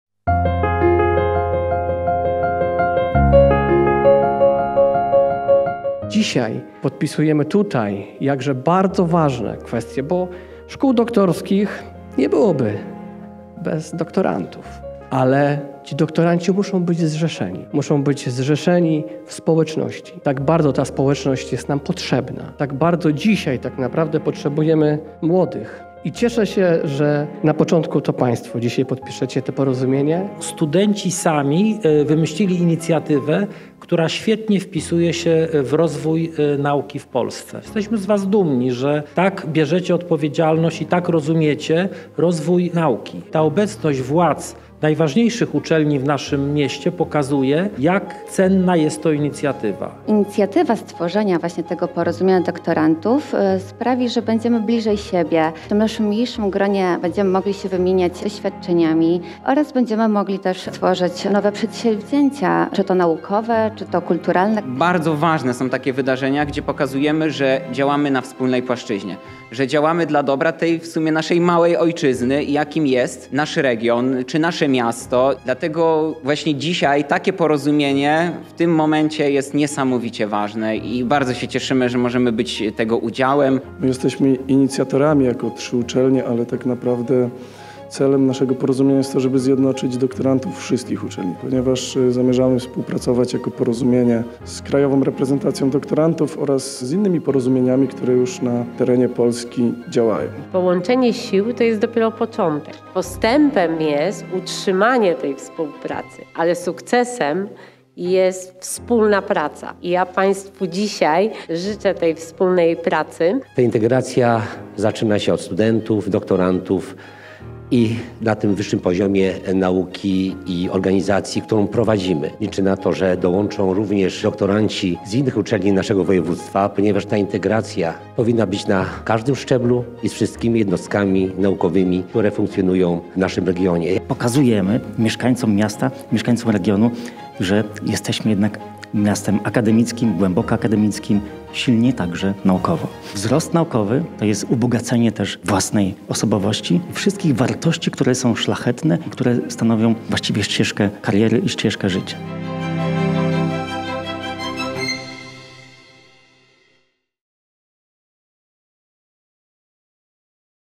Zachodniopomorskie Porozumienie Doktorantów
12 maja przewodniczący trzech samorządów doktorantów złożyli podpisy pod dokumentem inicjatywy integrującej środowiska doktorantów zachodniopomorskich uczelni.
W spotkaniu udział wzięły władze rektorskie szczecińskich uczelni: prof. dr hab. Waldemar Tarczyński, Rektor Uniwersytetu Szczecińskiego, prof. dr hab. n. med. Bogusław Machaliński, Rektor Pomorskiego Uniwersytetu Medycznego oraz dr hab. Jacek Wróbel prof. ZUT, Rektor Zachodniopomorskiego Uniwersytetu Technologicznego.

Produkcja: UNIWIZJA – telewizja Uniwersytetu Szczecińskiego.